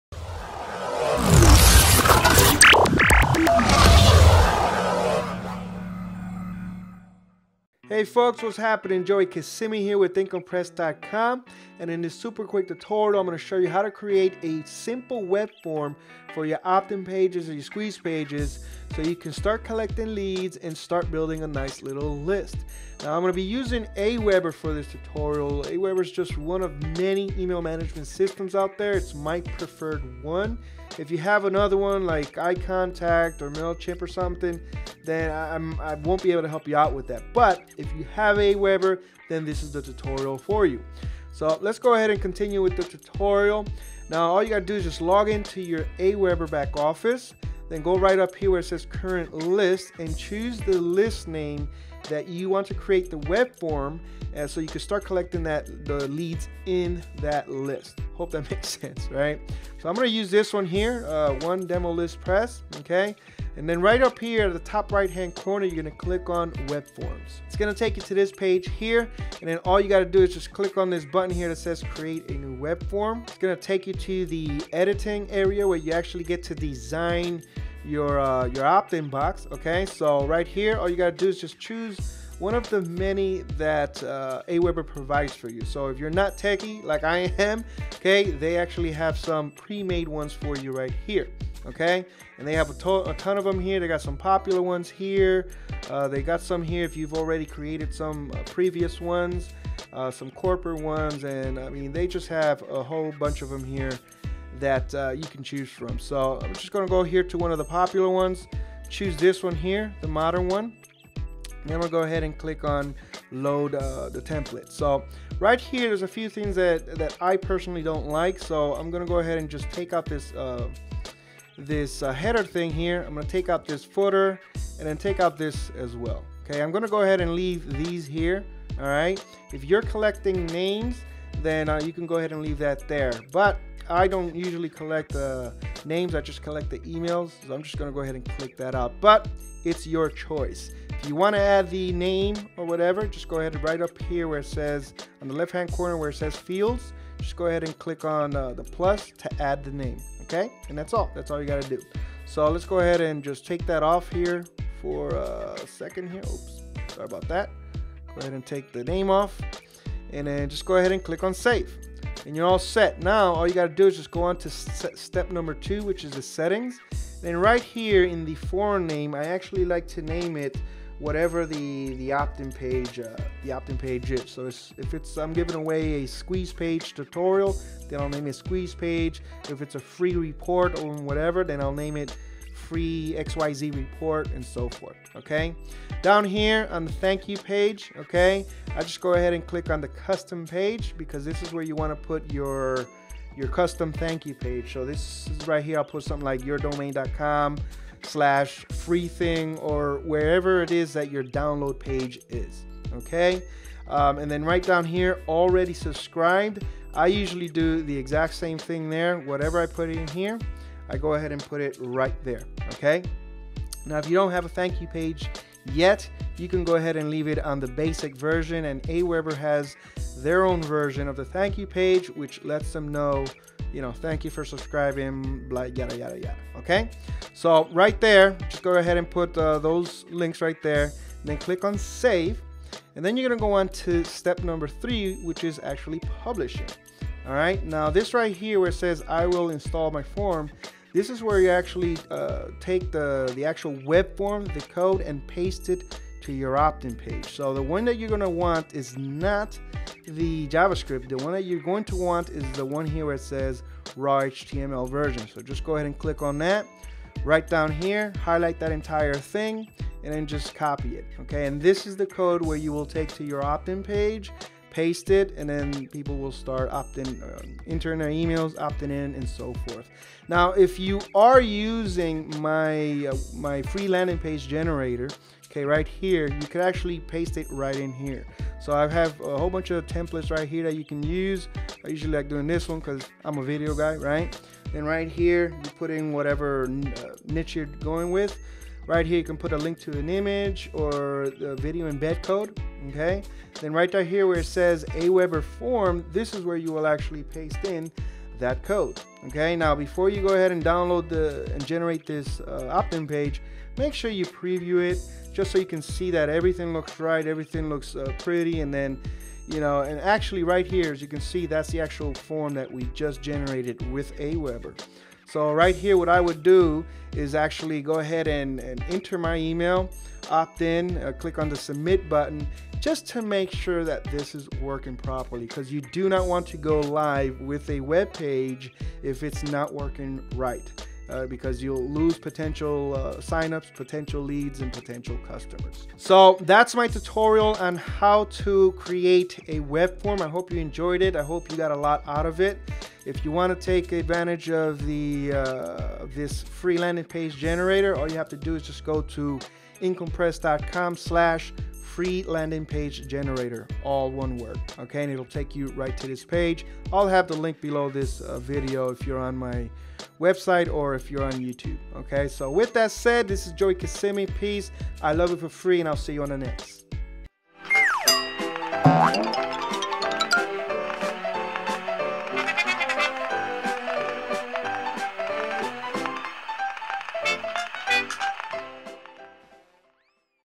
How to Create a Webform in Aweber for your Optin Page
- A quick tutorial on how to create a webform for your optin page, squeeze page, landing page, or capture page.

This is also know as an optin form.